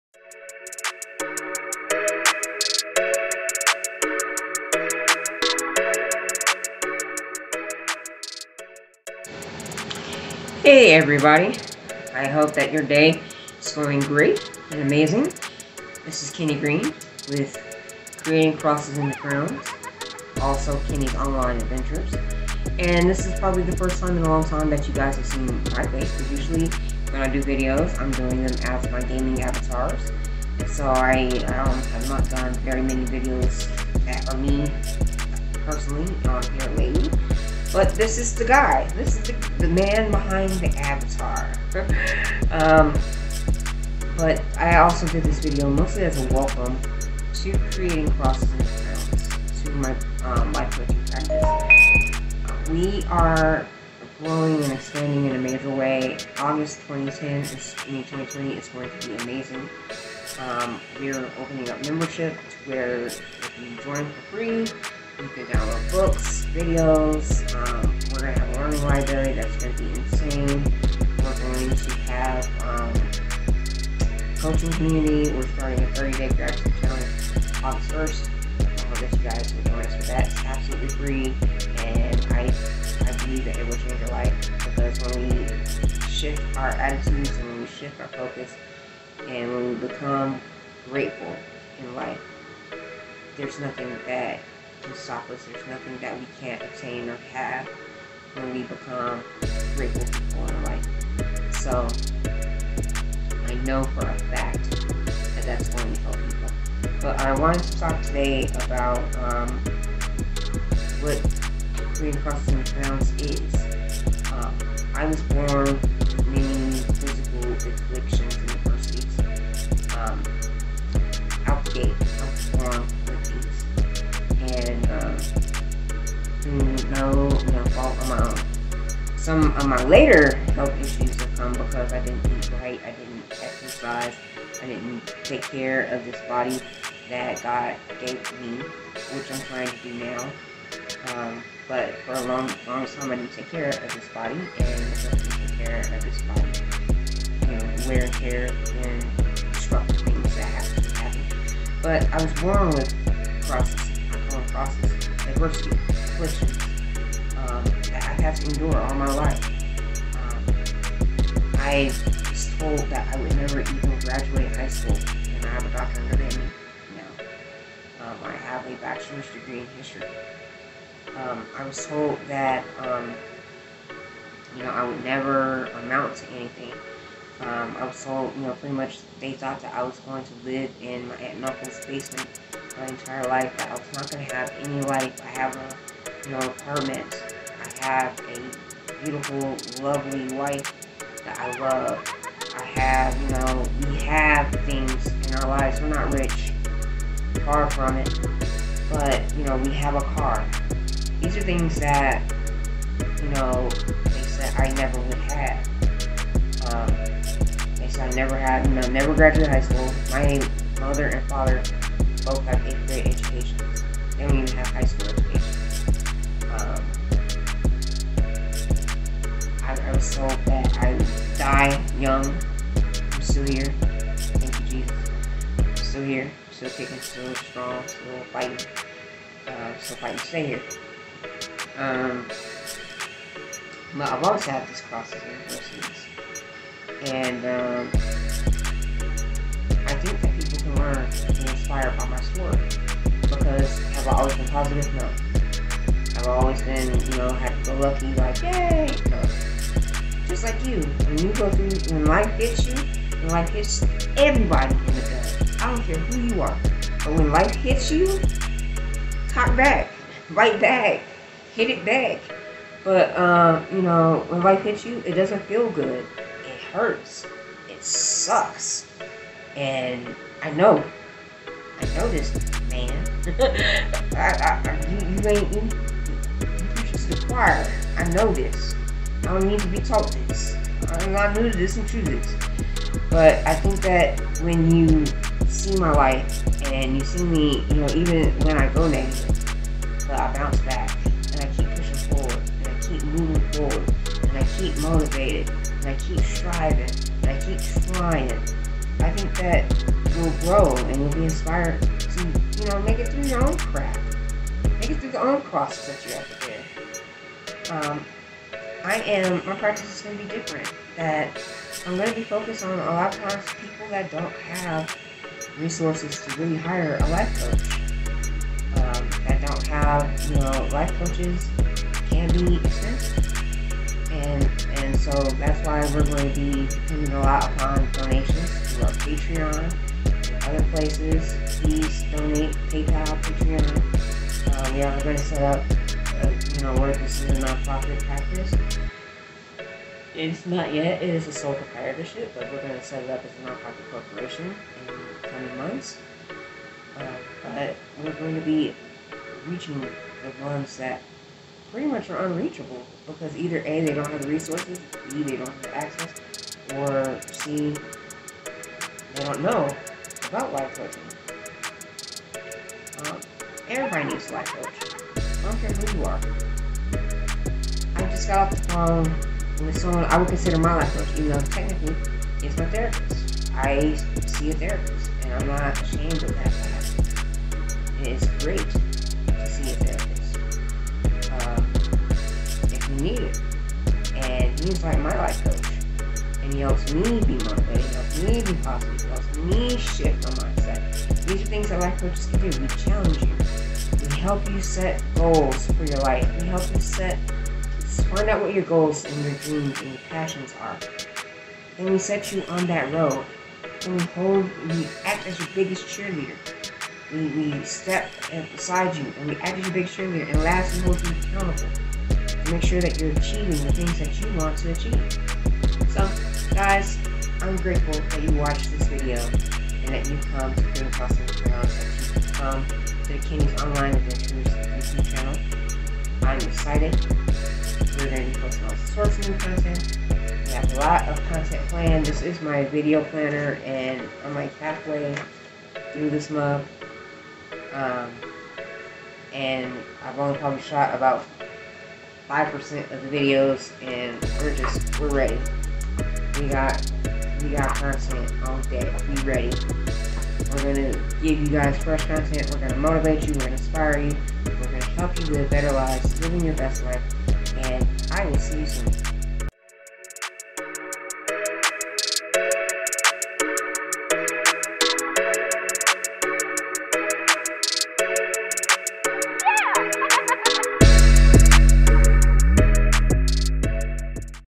Why I Became A Life Coach
Greetings,

On Today's Vlog Upload I wanted to talk about what has motivated me to become a life coach, and why life coaches are needed. I also go into what Creating Crosses Into Crowns is all about and what we hope to accomplish! It's my first video in a long time so I hope you guys enjoy it!